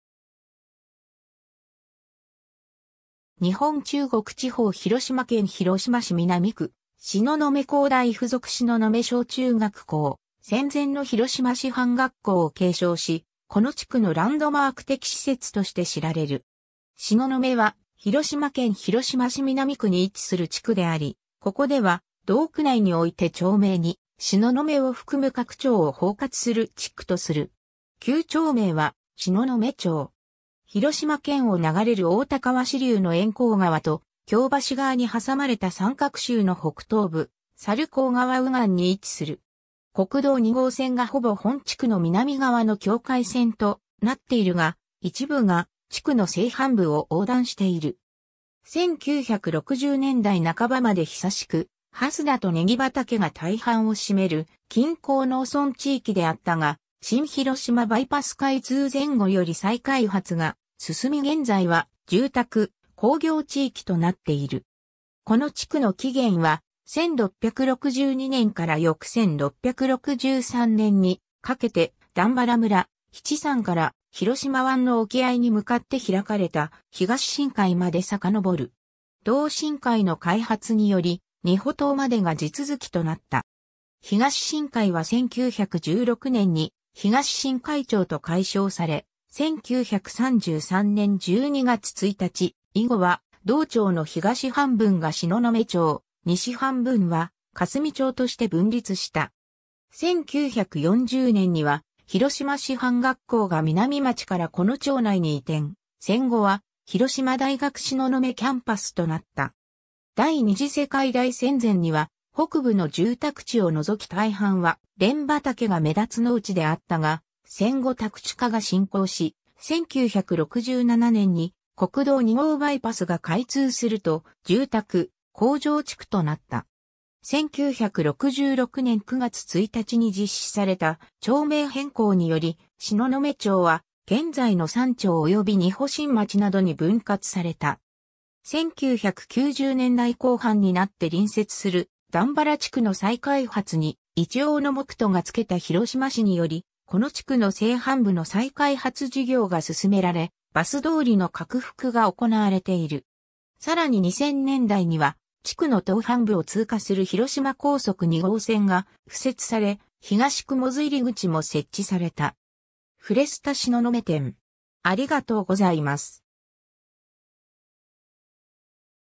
東雲 (広島市)
日本 」 中国地方 」 広島県 」 広島市 」 南区 (広島市) 」 東雲 (広島市) 広大附属東雲小・中学校 / 戦前の広島師範学校を継承し、この地区のランドマーク的施設として知られる。
 東雲（しののめ）は、広島県広島市南区に位置する地区であり、ここでは同区内において町名に「東雲」を含む各町を包括する地区とする。
旧町名は「東雲町」（ - ちょう）。
 広島県を流れる太田川支流の猿猴川と京橋川に挟まれた三角州の北東部、猿候川右岸に位置する。
 国道2号線（新広島バイパス）がほぼ本地区の南側の境界線となっているが、一部が地区の西半部（東雲本町）を横断している。
1960年代半ばまで久しく蓮田とネギ畑が大半を占める近郊農村地域であったが、新広島バイパス開通前後より再開発が進み現在は住宅・工業地域となっている。
 この地区の起源は、1662年（寛文2年）から翌1663年（同3年）にかけて段原村・比治山から広島湾の沖合に向かって開かれた「東新開」（ひがししんがい / 「仁保島東新開」とも）まで遡る。
同新開の開発により、仁保島（現在の黄金山）までが地続きとなった。
 東新開は1916年（大正5年）に東新開町と改称され、1933年（昭和8年）12月1日以後は同町の東半分が東雲町、西半分は霞町として分立した。
1940年には、広島師範学校が皆実町（現在は比治山本町内）からこの町内に移転、戦後は広島大学東雲キャンパス（学校教育学部および同附属東雲小学校・中学校）となった（学部は2004年に廃止されたものの、附属小・中は現在まで存続している）。
 第二次世界大戦前には北部の住宅地を除き大半は蓮畑が目立つ農地であったが、戦後宅地化が進行し、1967年に国道2号バイパス（新広島バイパス）が開通すると住宅・工場地区となった。
1966年9月1日に実施された町名変更により、東雲町は現在の3町および仁保新町（一・二丁目）などに分割された。
1990年代後半になって隣接する段原地区の再開発に一応の目途がつけた広島市により、この地区の西半部（上東雲町および東雲本町）の再開発事業が進められバス通りの拡幅が行われている。
さらに2000年代には地区の東半部（東雲3丁目）を通過する広島高速2号線が敷設され東雲出入口も設置された。
 フレスタ東雲店